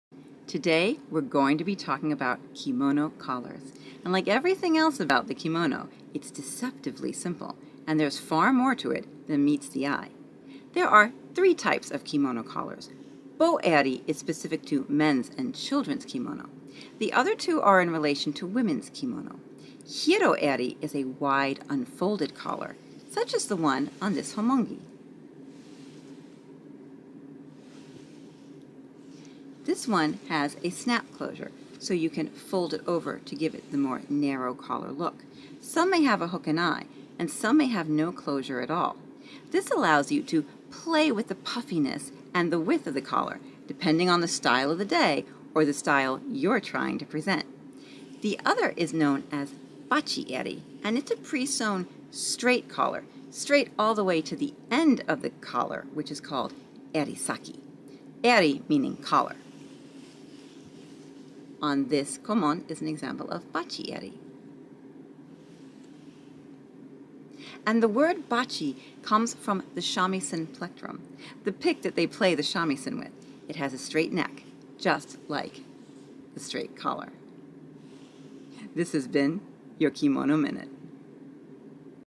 "The Kimono Minute" Kimono Collars Part One
The collar of the kimono is deceptively simple and anything but...

There are 3 types of kimono collar:

bo-eri -
Seen on men's and children's kimono
hiro-eri -
A wide unfolded collar
bach-eri -
A narrow, straight pre folded and sewn collar